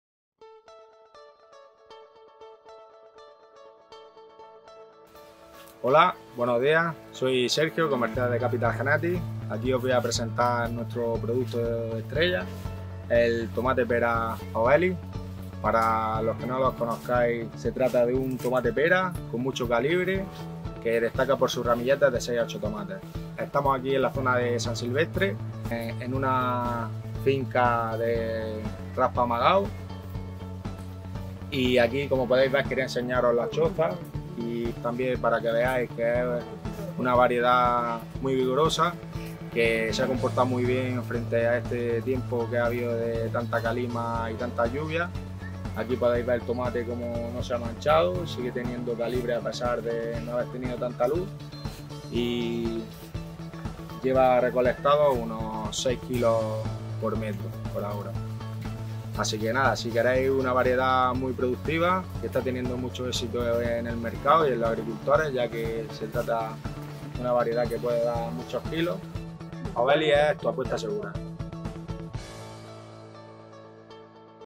Tomate Obelix - Sergio López Manzano
Hoy os presentamos a Sergio López Manzano, de nuestro equipo comercial, que nos habla de este tomate pera de gran calibre en esta finca en San Silvestre (Almería).

Obelix es una variedad de alta producción y con mucha adaptabilidad a diversos terrenos.

Tiene Resistencia HR: Fol:0,1/Va,Vd/ToMV y Resistencia IR: TYLCV y destaca por su gran tamaño, color intenso, brillo y firmeza.

Somos una empresa puntera en investigación agrícola y ofrecemos la mayor calidad en semillas hortícolas.